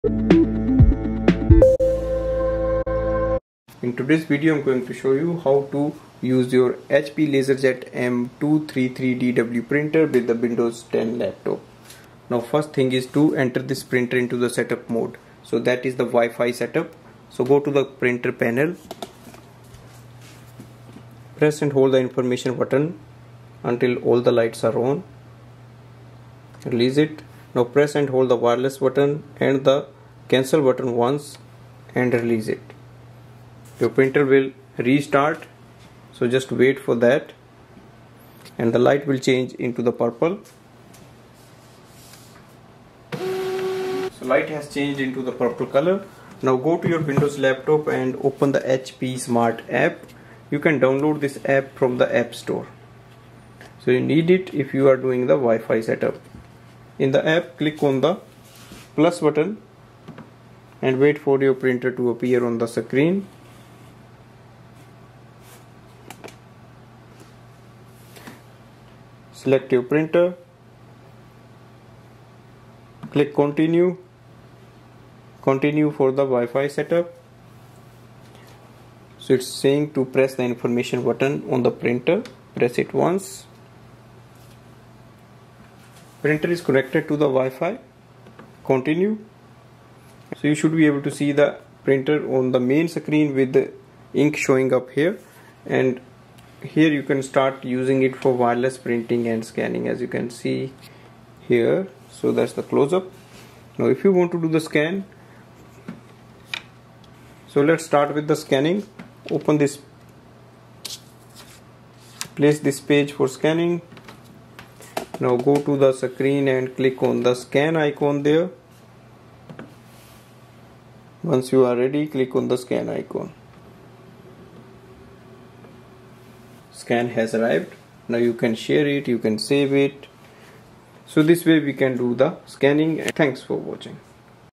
HP LaserJet MFP M233dw WiFi Setup Using a Windows Laptop, Wireless Scanning Video.
This video shows how to do the wireless setup of HP LaserJet M233dw printer using a windows 10 laptop for wireless scanning & printing.